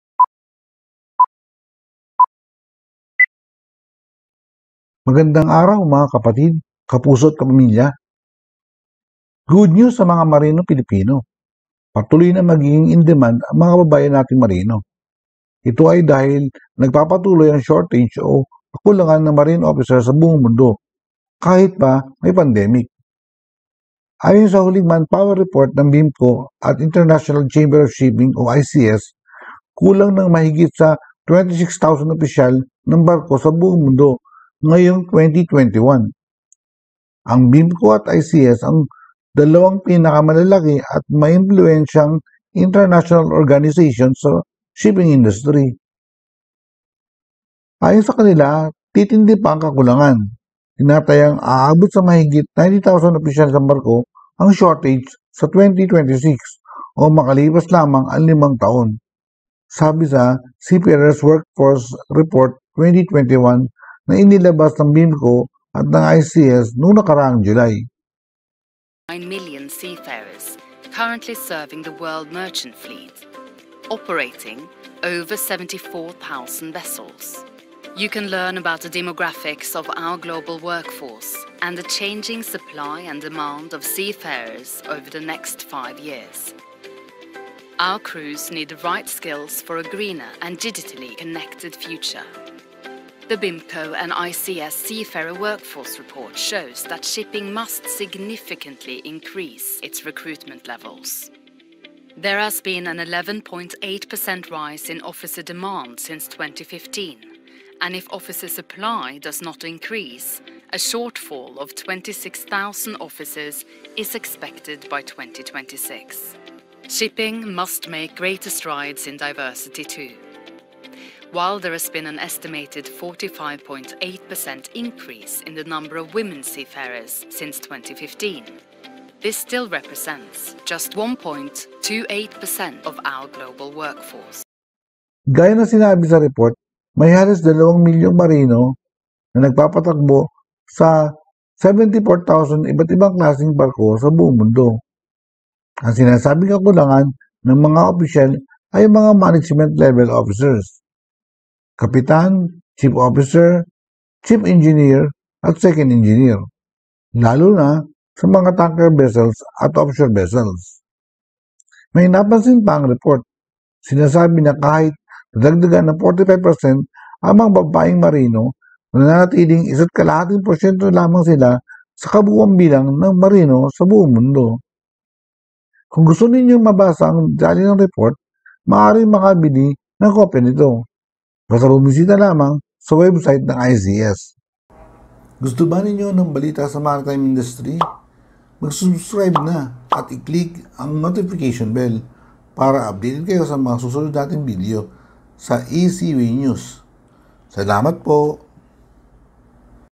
Strong demand for Filipinos officers to continue
Strong demand for Filipino officers for overseas employment is expected to continue as BIMCO and ICS projected a worldwide shortage of 90,000 officers until 2026, according to the Seafarer Workforce Report 2021.